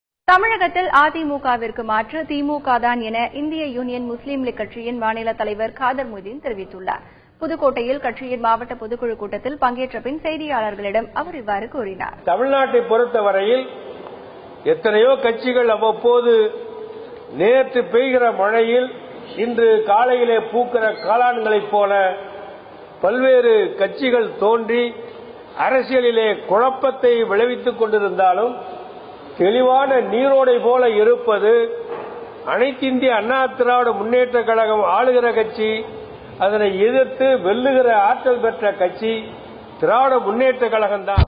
DMK is an only Alternative to AIADMK : Kader Mohideen, Indian Union Muslim League - Thnathi TV
DMK is an only Alternative to AIADMK : Kader Mohideen,  Indian Union Muslim League - Thanthi TV